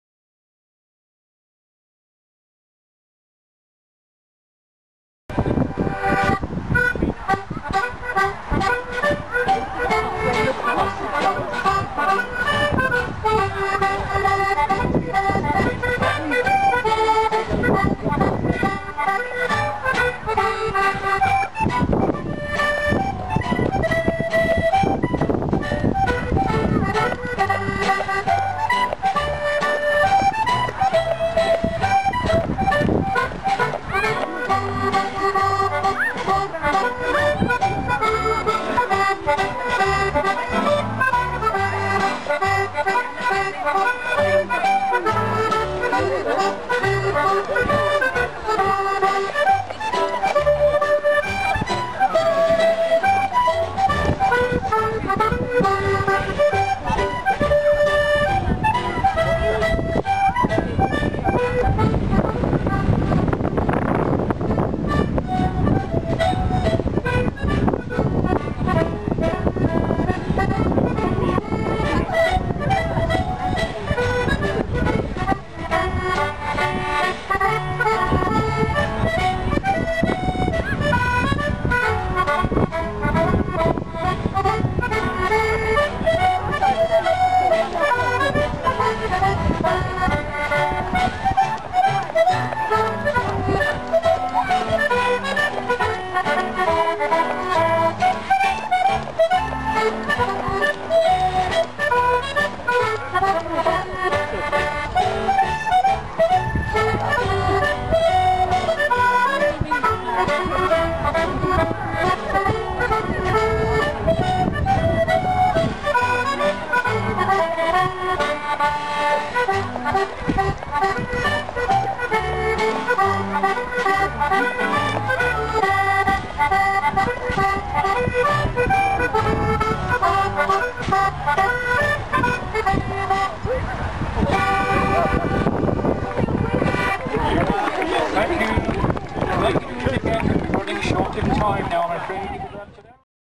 The Shepherds Crook - White Rose Festival - Leeds, 9th July 2011
Nicola Scott, Henry Wilkinson and Elizabeth Cooper from the Birmingham RSCDS perform "The Shepherd's Crook" at the White Rose Festival in Leeds on the 9th July 2011